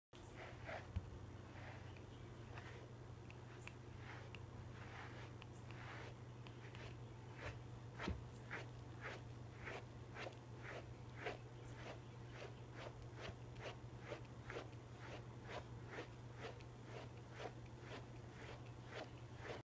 Funny Puppy Loves to be Groomed
This funny puppy tilts his head back and shakes his butt while getting groomed.